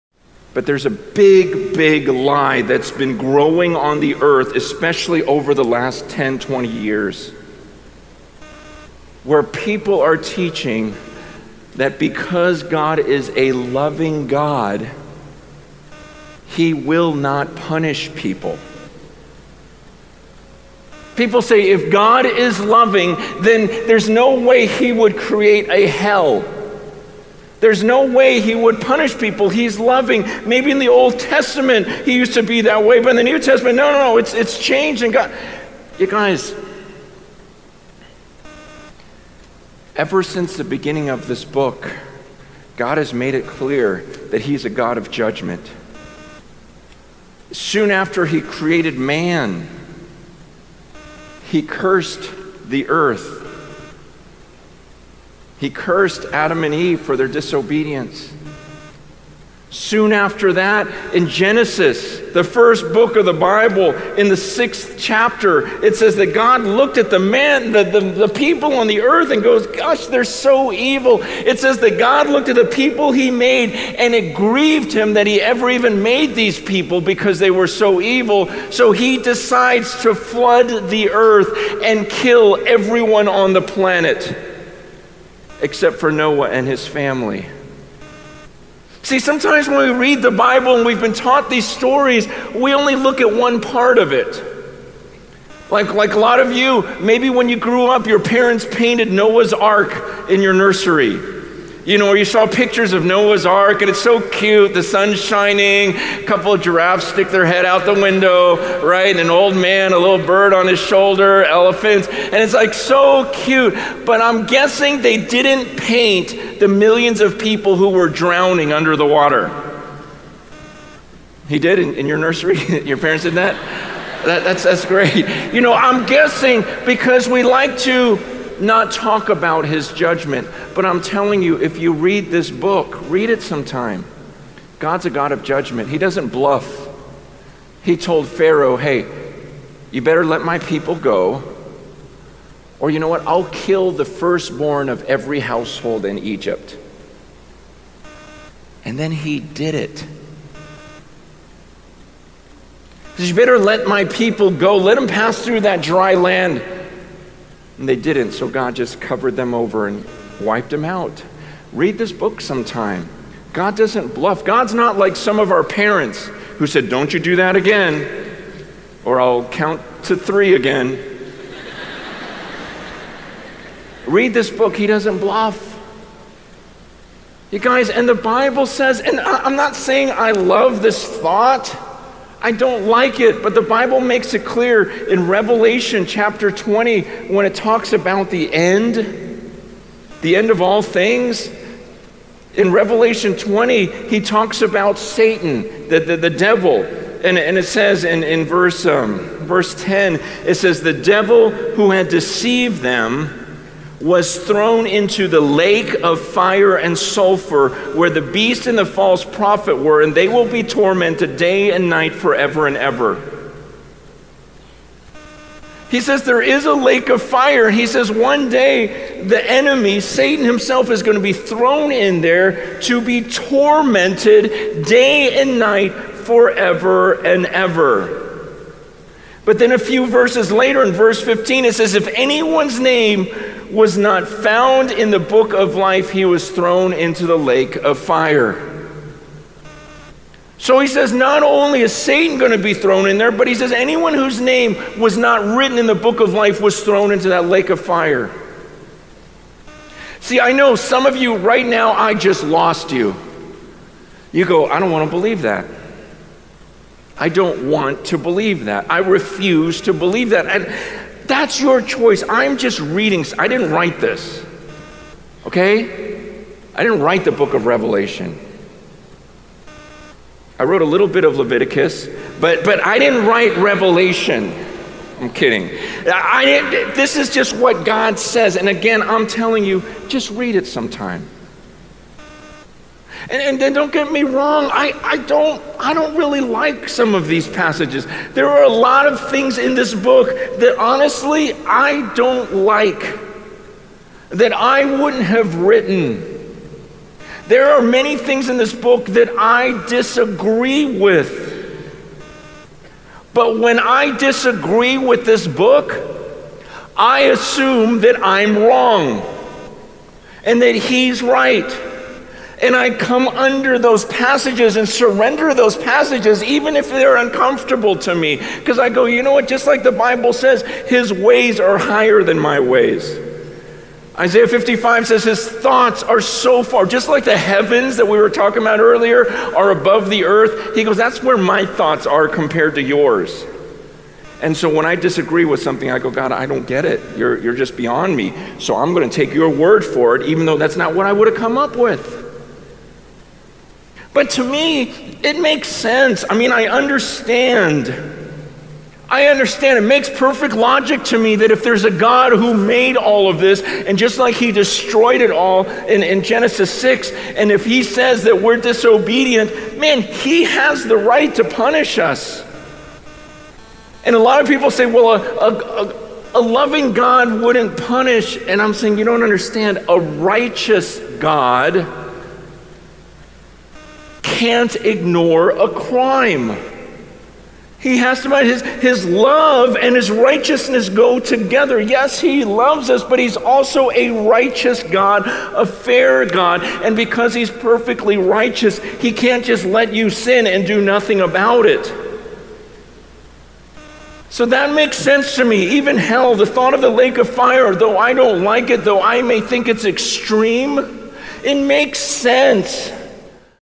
How can a loving God send people to hell? Why did God make hell? - Francis Chan
Francis Chan - How can a loving God send people to hell? Why did God make hell?
In this video Francis Chan explains why God created hell and for what purpose.